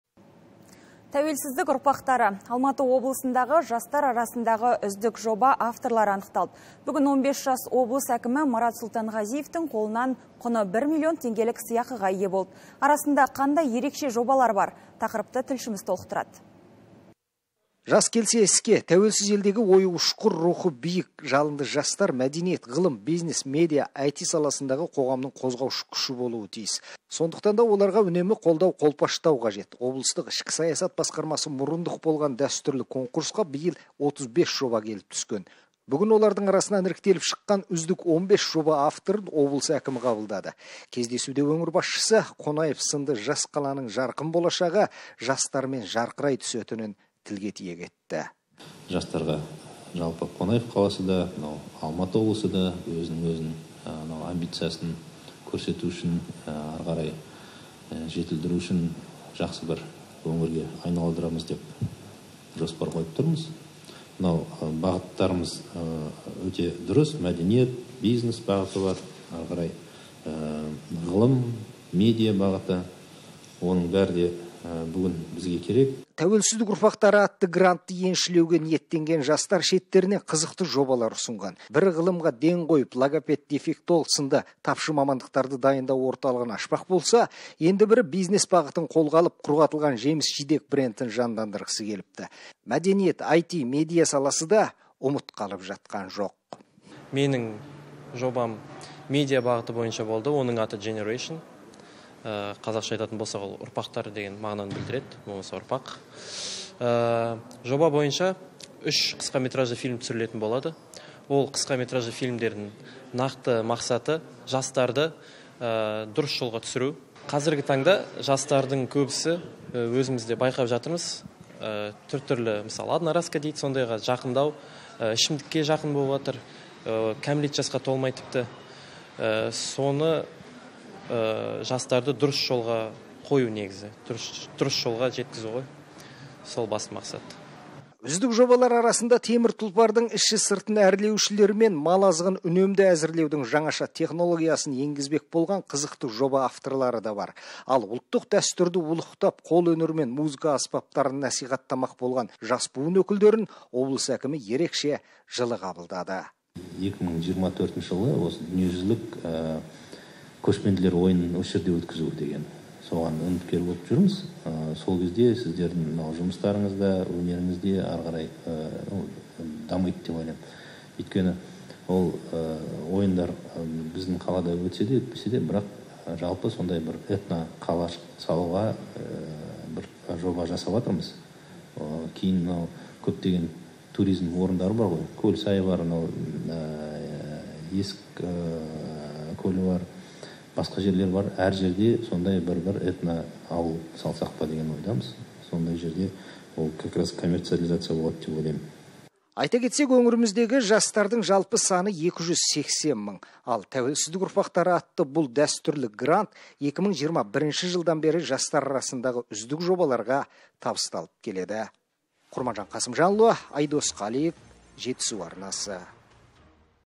Жастар арасында үздік жоба авторлары анықталды
Тәуелсіздік ұрпақтары! Алматы облысындағы жастар арасындағы үздік жоба авторлары анықұталды. Бүгін 15 жас облыс әкімі Марат Сұлтанғазиевтің қолынан құны бір милион теңгелік сый ақыға ие болды. Арасында қандай еркше жобалар бар?